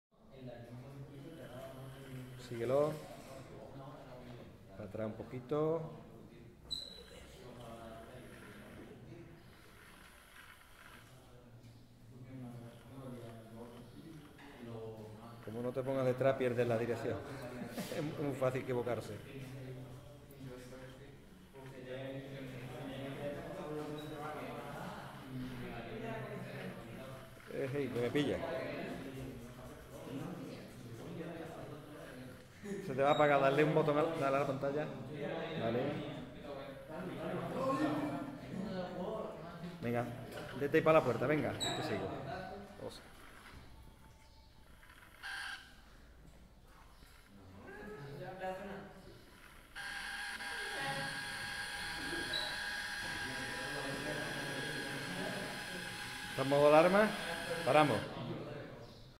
Joystick para coche por Bluetooth - Demo
Ampliación de Proyecto
Joystick para el coche controlado por Bluetooth

Se realiza una demostración de los movimientos del coche realizando unas cortas maniobras. Puede verse cómo la tableta se inclina según la posición del mando realizando el movimiento deseado.